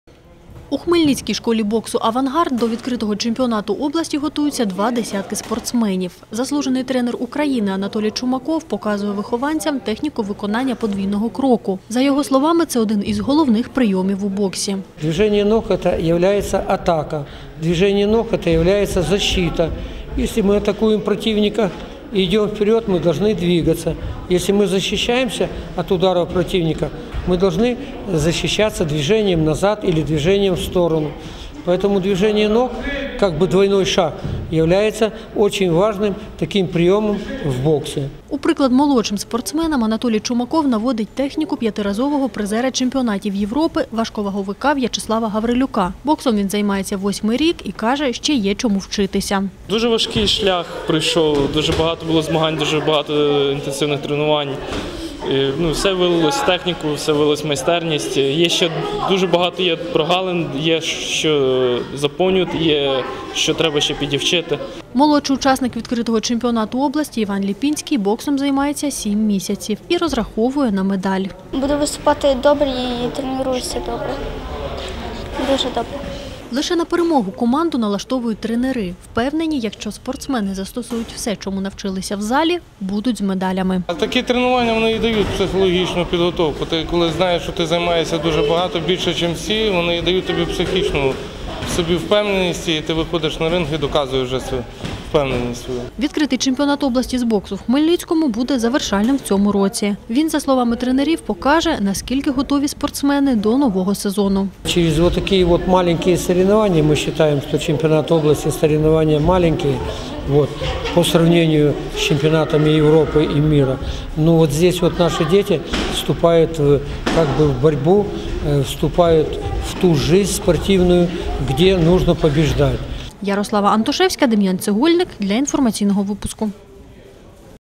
Два дні змагатимуться боксери області в Хмельницькому
Відкритий чемпіонат області з боксу сьогодні стартує в Хмельницькому. Два дні на ринзі змагатимуться більше сотні спортсменів. Таку кількість учасників попередньо назвали в обласній федерації боксу. Розраховують організатори і на приїзд боксерів з Молдови. Як до чемпіонату готувалися хмельничани - бачили наші кореспонденти.